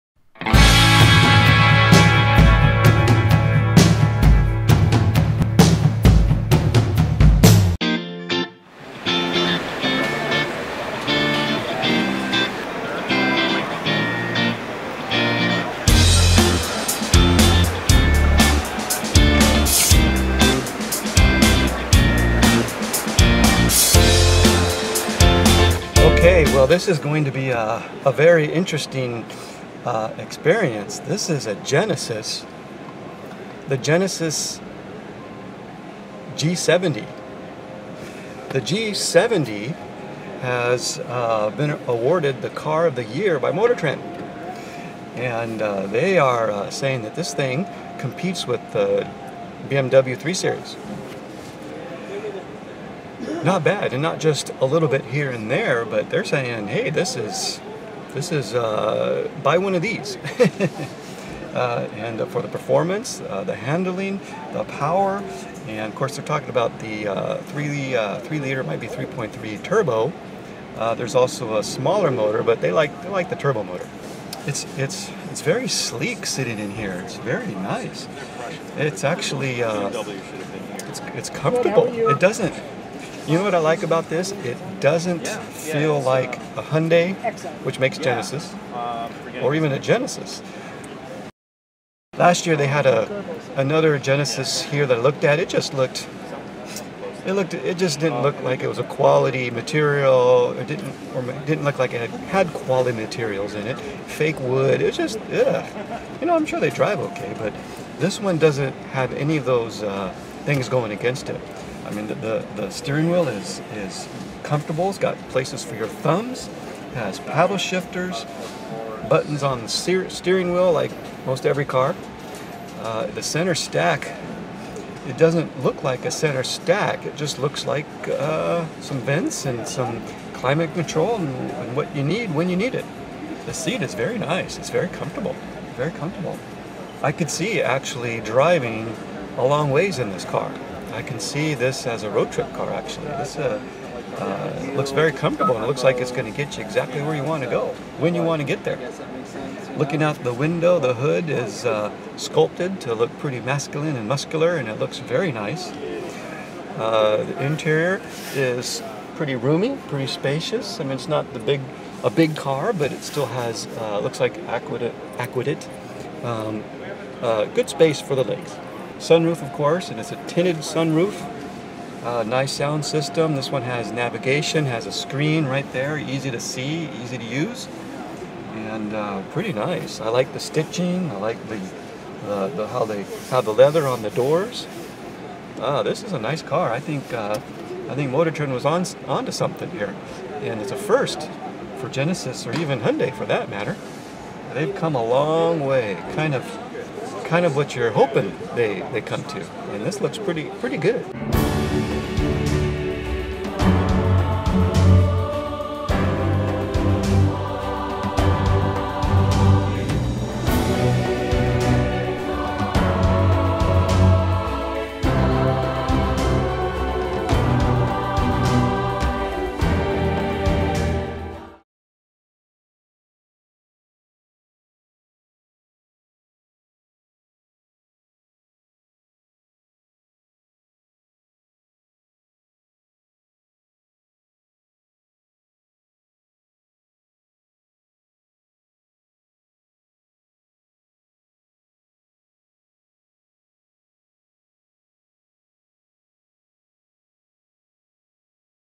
The 2019 Genesis G70 - First Look - LA Auto Show - Part 2
This is the new 2019 Genesis G70 at the LA Auto show.  This car won
 the 2019 Motortrend car of the year.  It is sleak, smooth, powerful car that looks great.  It is a direct competitor to the BMW 3 series.  This is a first for Genesis and Hyundai.